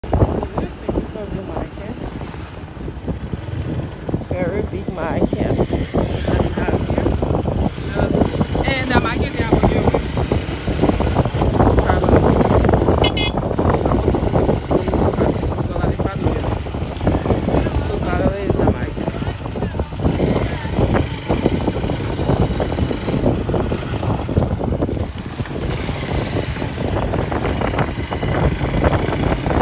Americo Liberian from Caribbean immigrants home town in Liberia Africa oldest republic
OMG you won’t believe what I found in Crozierville the caribbean immigrant town in Liberia